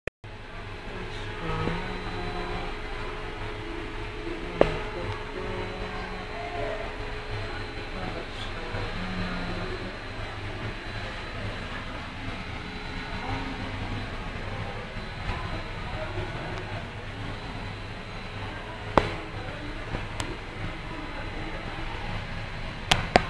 First Class A/C Washroom
Have a look how this Metalled walle bathroom of ICF 1st ac bathroom looks like...!!!